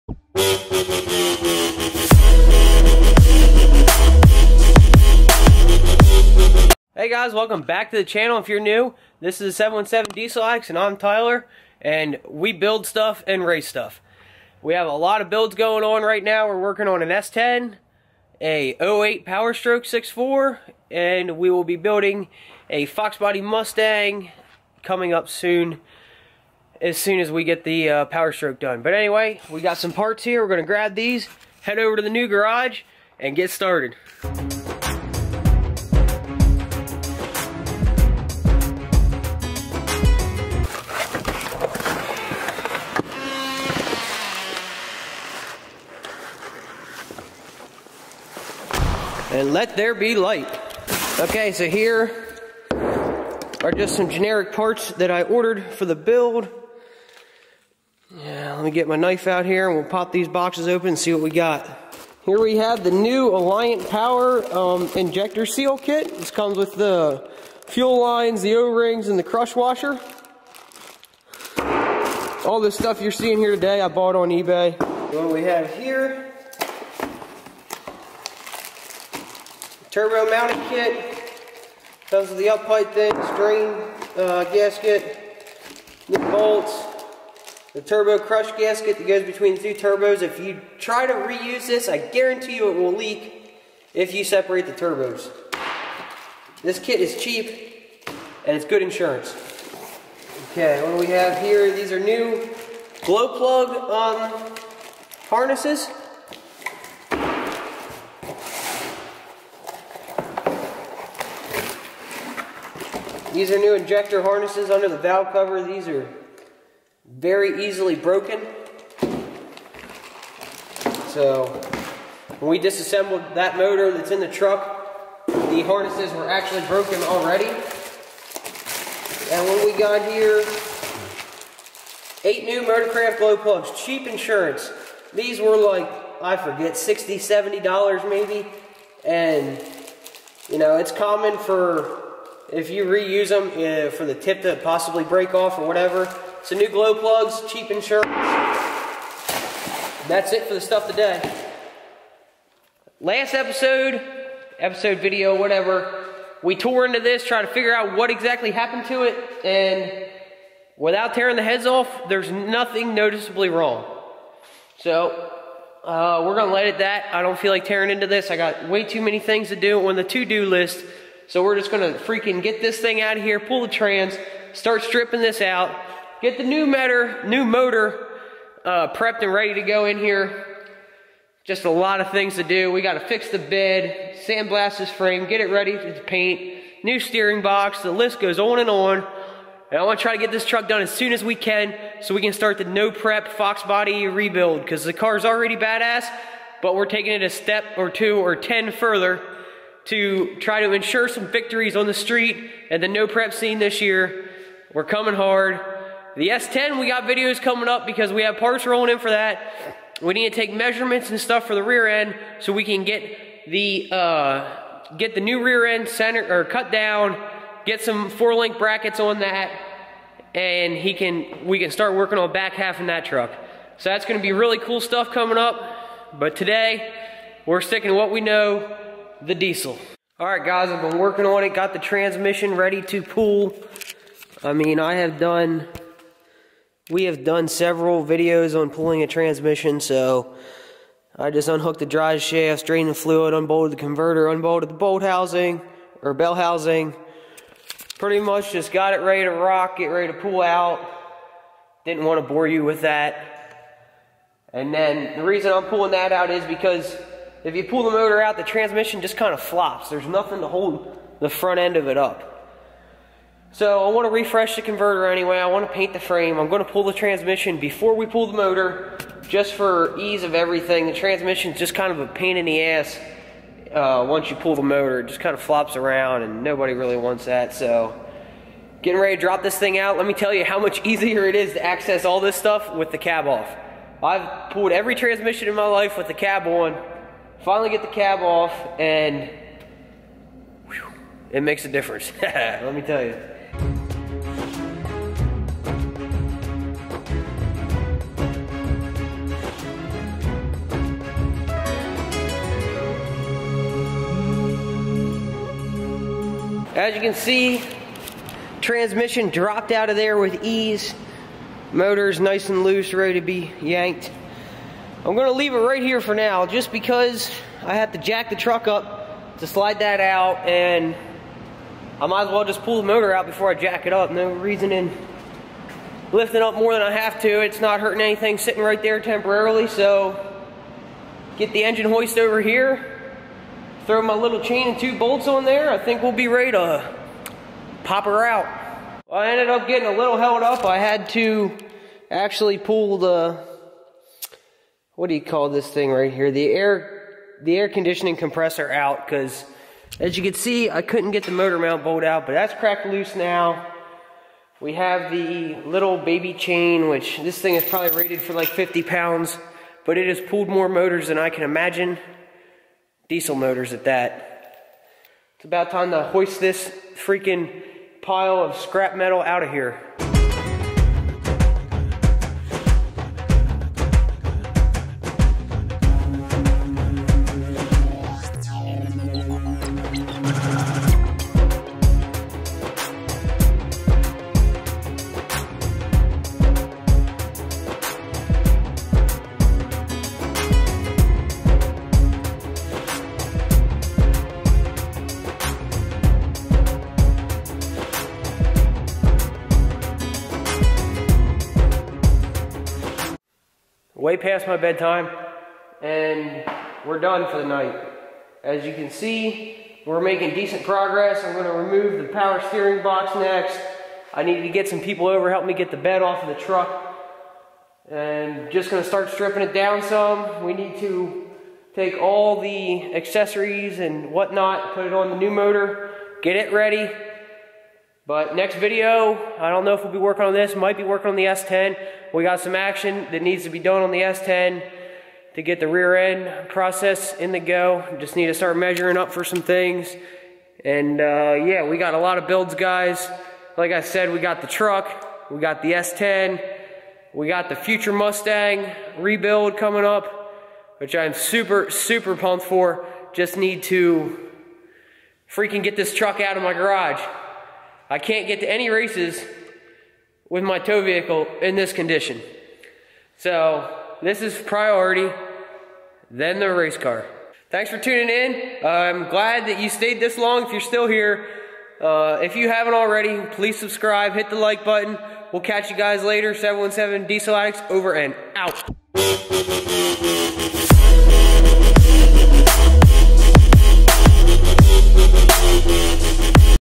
Stripping my 6.4 powerstroke down to nothing !
We take the 6.4 powerstroke down to the bare frame.